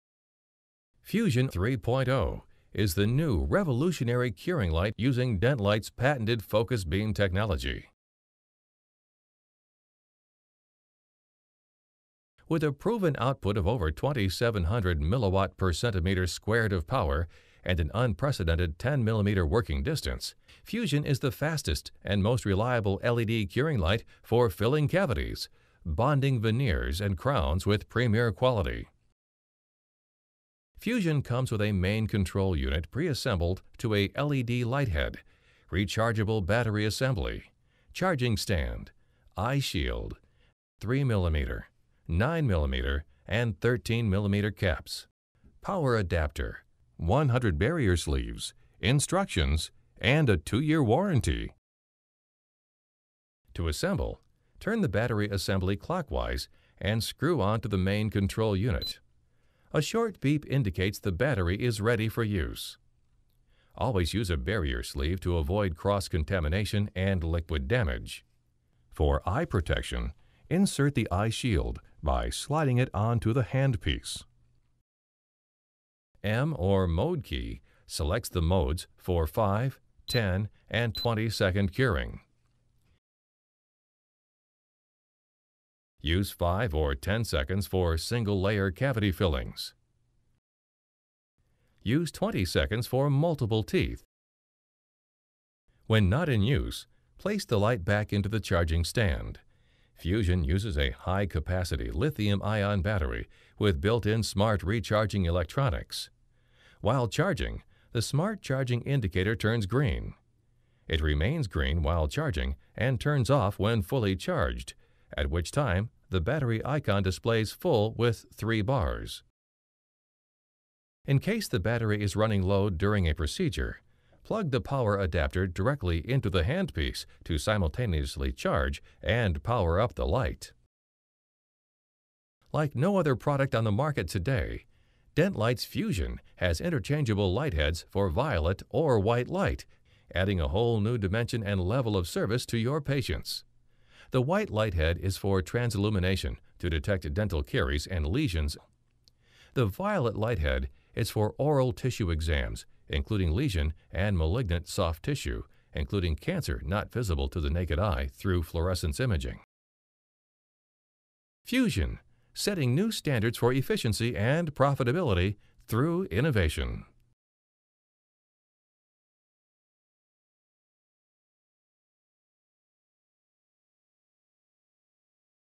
DentLight FUSION dental curing light
Curing with high-power Focused Beam

The most highly rated dental curing light

Incredible size and unmatched durability
Amazing to have an LCD in a light this small
Solid metal construction with sturdy built-in for shock proof durability
Modular, upgradeable and easy service
Lowest profile light probe that allows access anywhere
Cordless or corded with replaceable battery

You've got the POWER!!!
Plasma-equivalent cure speed
2700mW/cm2 focused, low dispersion beam ensures secure/fast bonding
5 second cures of all composites
Time Savings $30,000 per year

Upgradable to oral cancer exam light, dental caries detection light, composite cleaning/removal and bleaching light